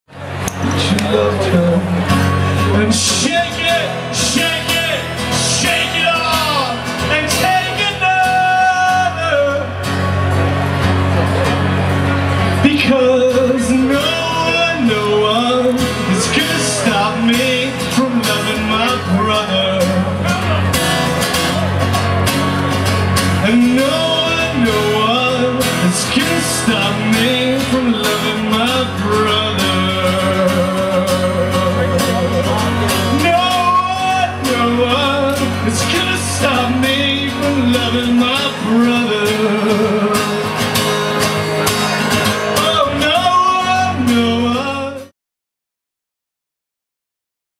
SXSW '12: Will Sheff
Will Sheff performs at The Belmont during SXSW '12.